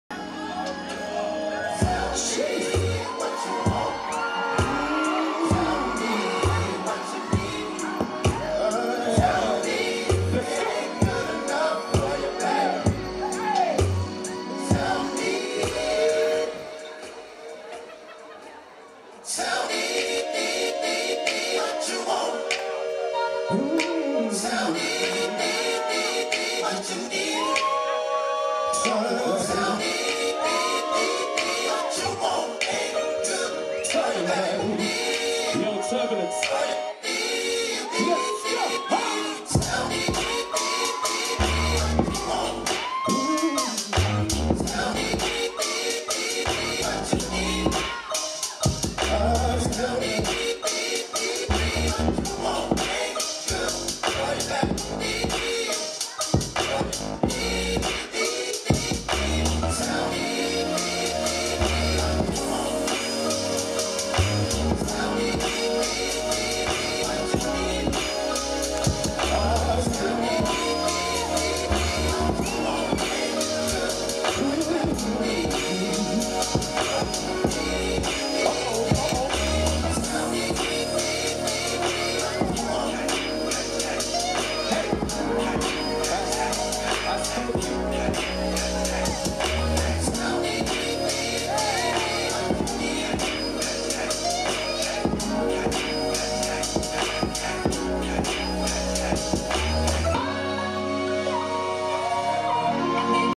Turbo | UDO British Championships 2017 | Judges Showcase
Judges showcase by Turbo at the 2017 UDO British Championships.

Witness incredible performances and showcases at the 2018 British Championships.